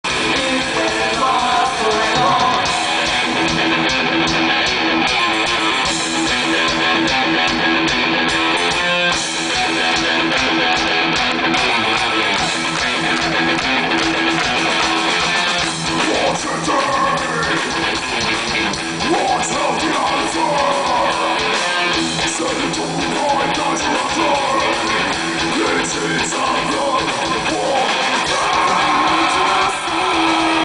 Battlelore MFVF5 (2)
Battlelore live @ Metal Female Voices Fest 5, Oktoberhallen, Wieze